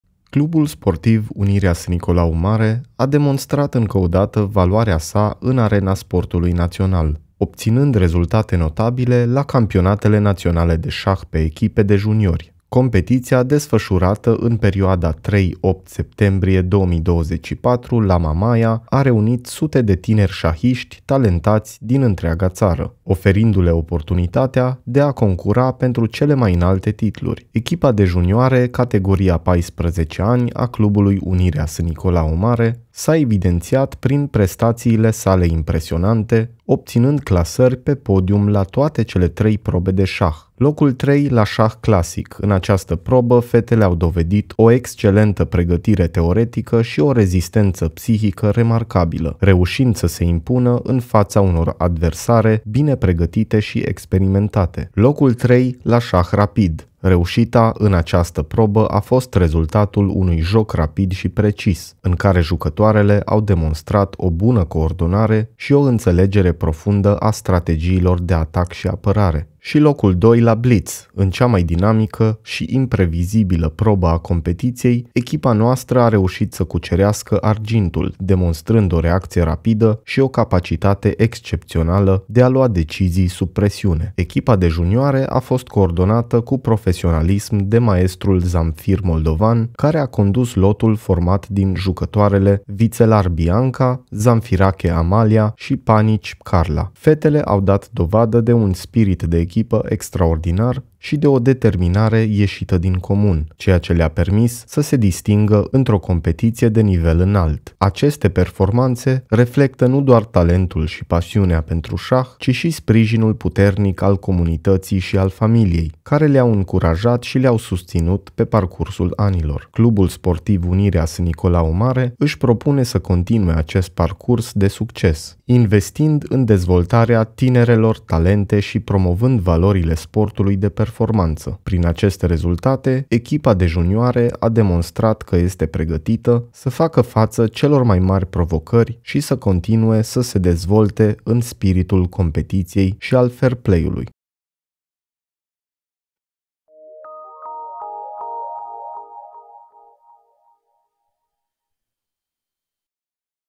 Performanță de excepție a echipei de șah a Clubului Unirea Sânnicolau Mare
Clubul Sportiv Unirea Sânnicolau Mare a demonstrat încă o dată valoarea sa în arena sportului național, obținând rezultate notabile la Campionatele Naționale de Șah pe Echipe de Juniori. Competiția, desfășurată în perioada 3-8 septembrie 2024 la Mamaia, a reunit sute de tineri șahiști talentați din întreaga țară, oferindu-le oportunitatea de a concura pentru cele mai înalte titluri.